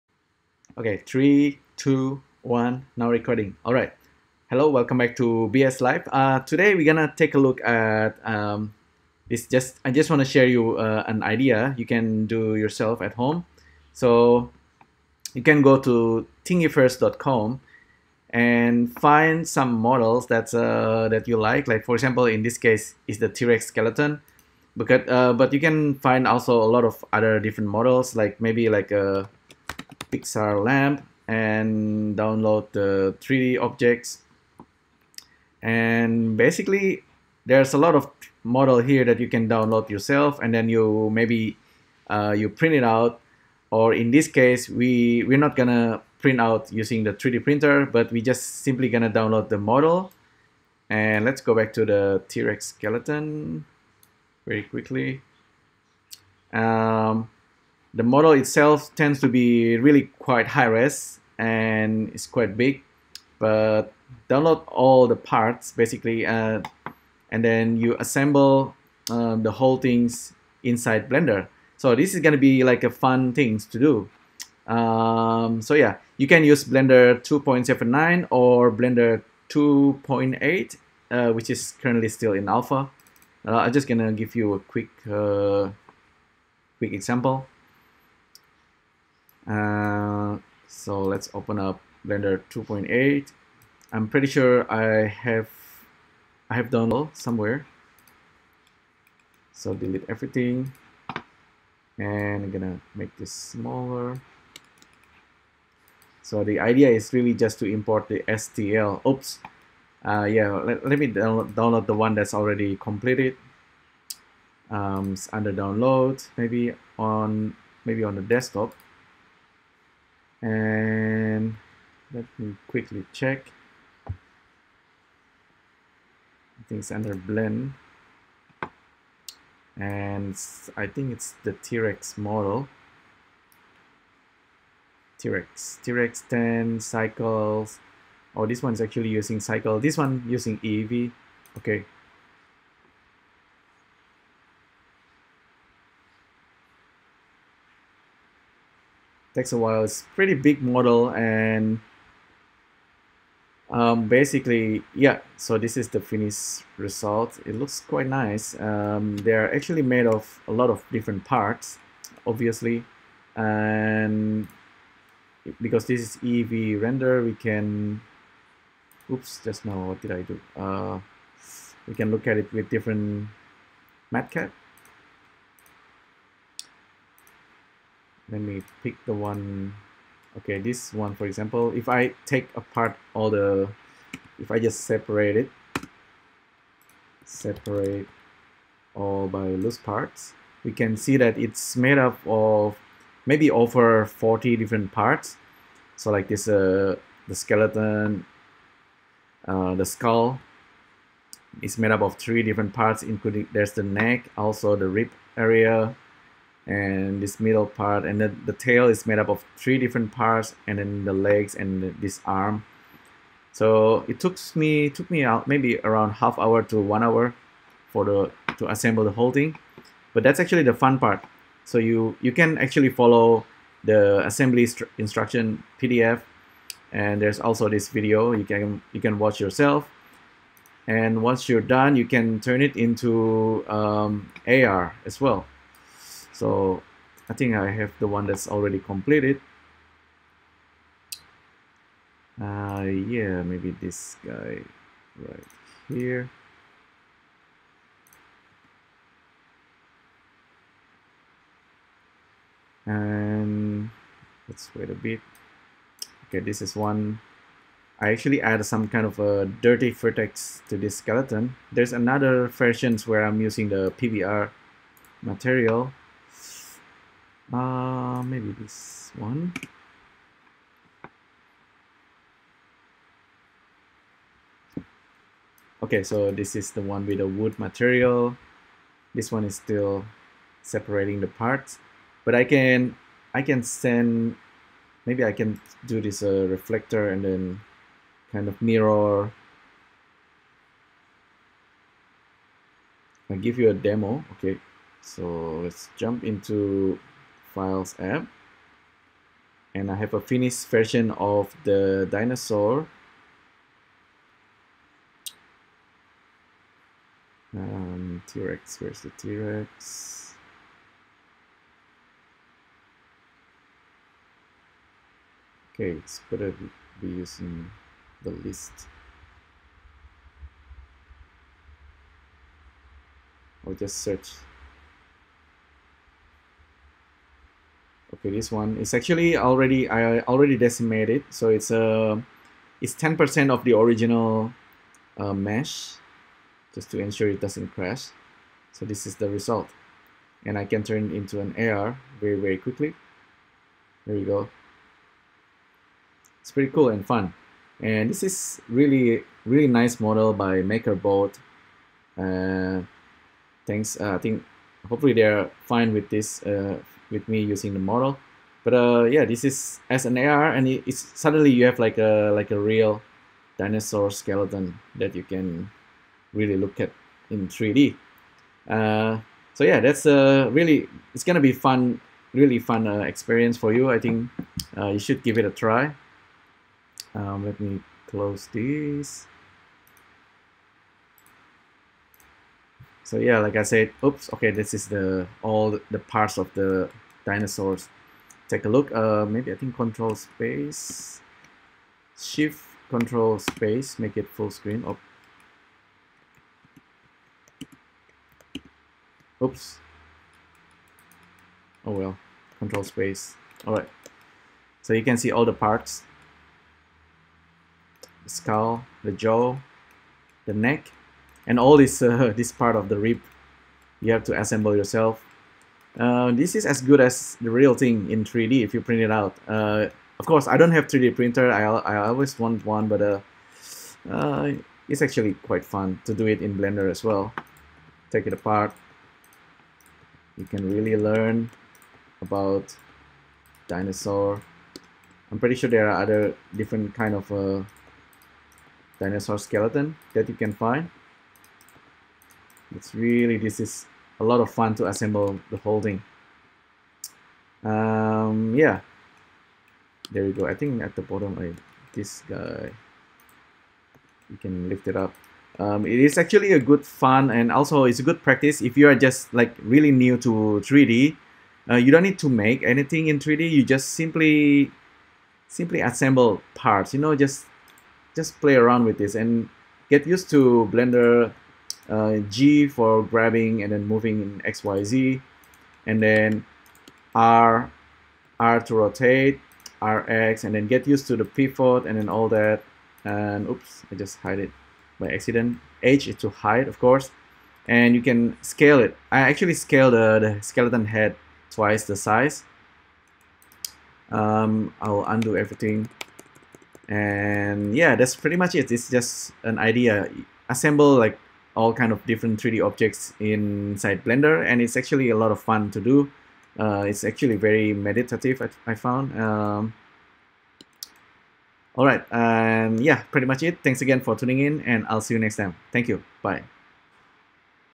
BSLIVE 0012 / Using Blender 3D to assemble T Rex Skeleton Model for AR
In this BS LIVE, I am going to share an idea that you can do using Blender 3D. Basically go to THINGIVERSE, find real 3D model you like, download all the parts and assemble it inside Blender.

I am showing you the final result here, but I actually started from scratch, the whole process as below:
- Import STL
- Separate Parts and rename as needed
- Follow assembly PDF
- Slowly attaching and building the full 3D model

Once done, you can export as OBJ and convert to USDZ for iOS 12 devices and use as Augmented Reality.